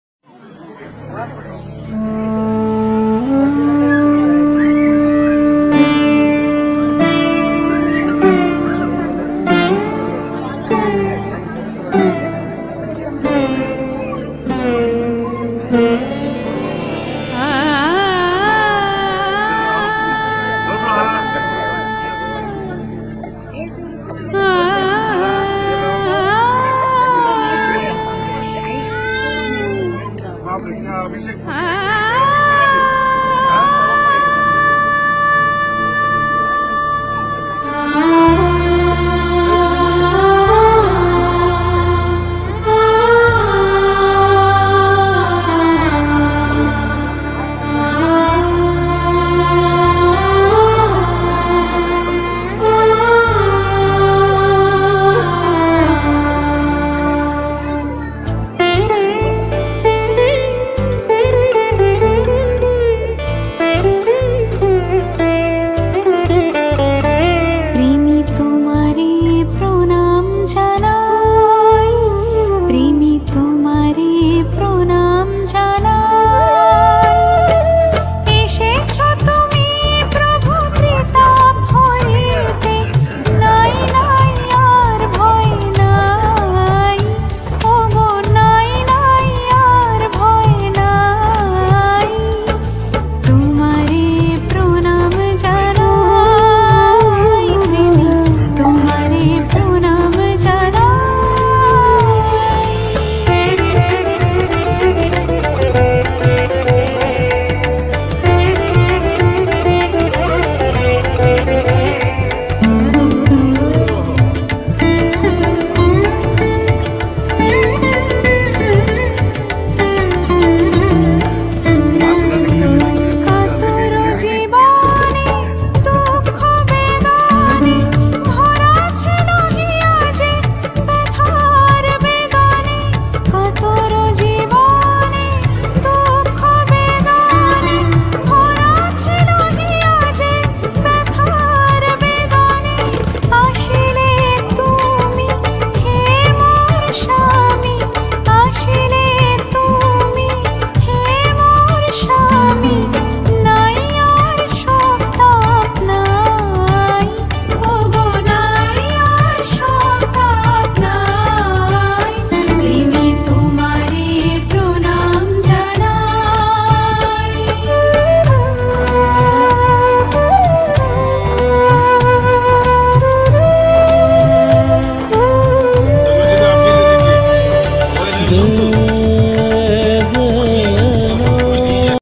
pujyapad Shree Shree Acharyadev Prasad Vitran karte hue apne bhakt jano ko 🙏🏻satsang_deoghar_
pujyapad Shree Shree Acharyadev Prasad Vitran karte hue apne bhakt jano ko 🙏🏻🙏🏻🙏🏻🙏🏻🙏🏻🙏🏻🙏🏻🙏🏻🙏🏻🙏🏻🙏🏻🙏🏻🙏🏻🙏🏻🙏🏻🙏🏻🙏🏻🙏🏻🙏🏻🙏🏻🙏🏻🙏🏻
Satsang_
Satsang_Bhajan_Thakurbari _Songs_Shorts_

joyguru#

@SATSANG ZONE

joyguru#